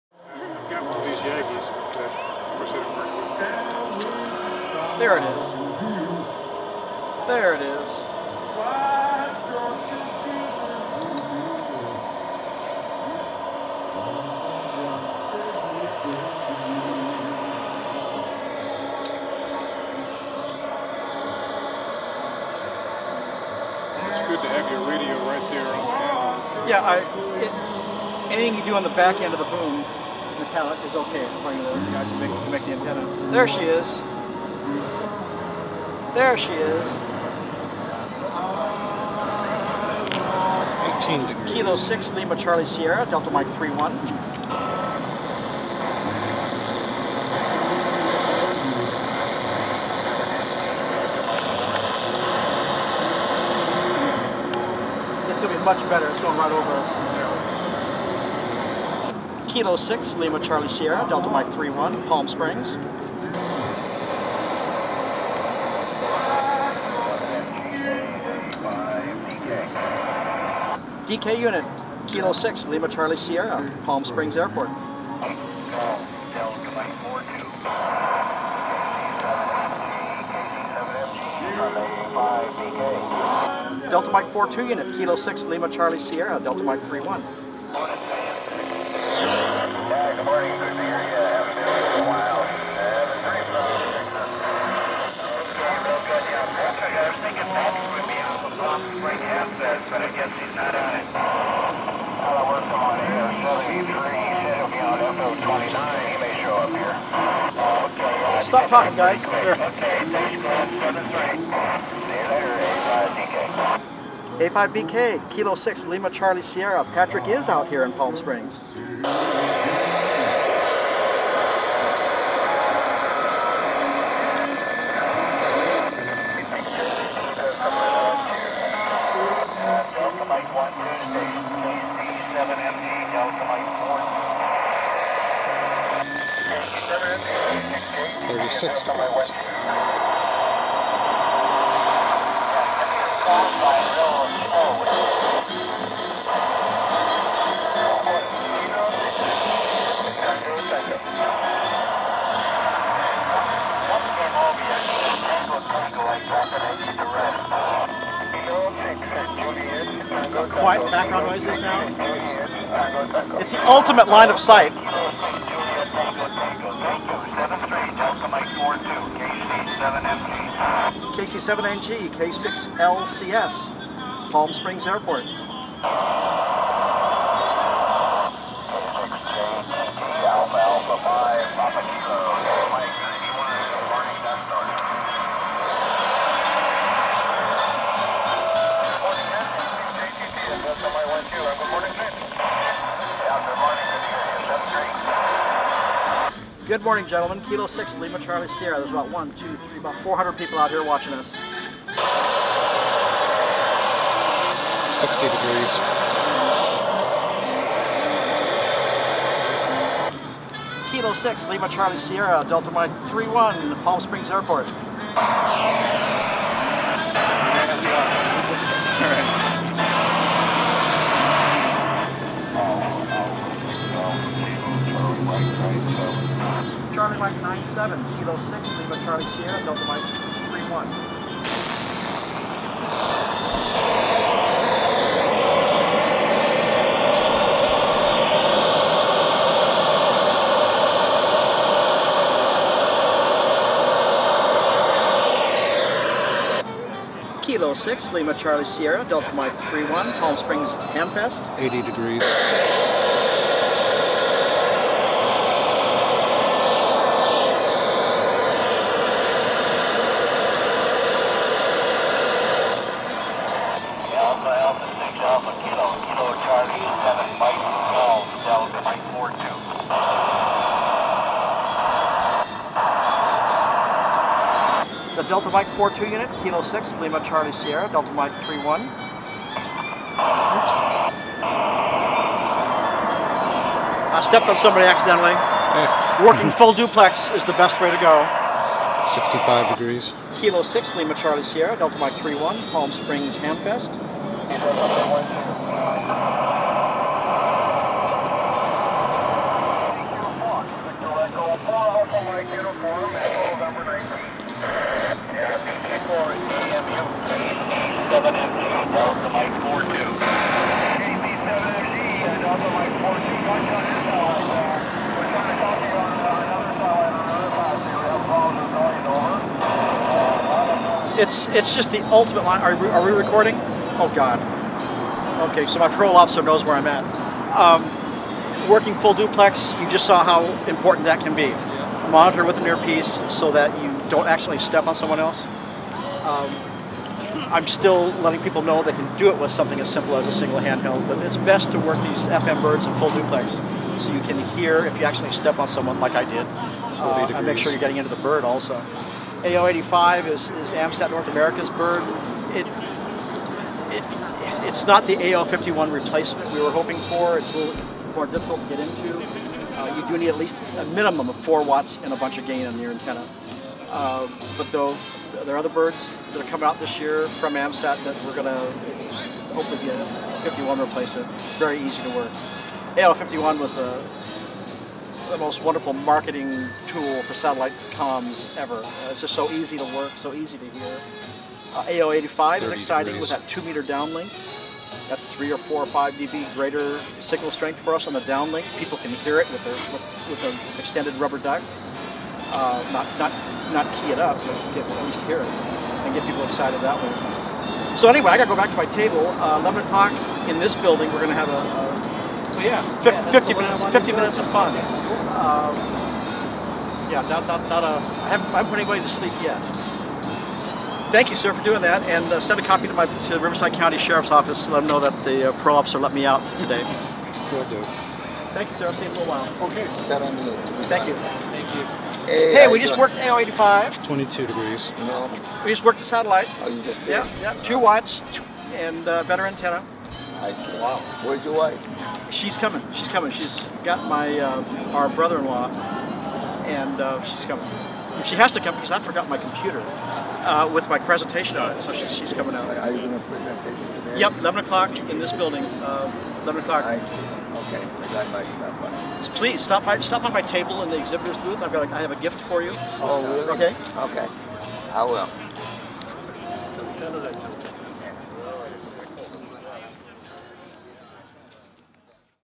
K6LCS Demonstrating Satellite Operation - AO-85
Palm Springs Hamfest 2017.  K6LCS demonstrating operation on FM satellites.  This was an  80° pass of AO-85.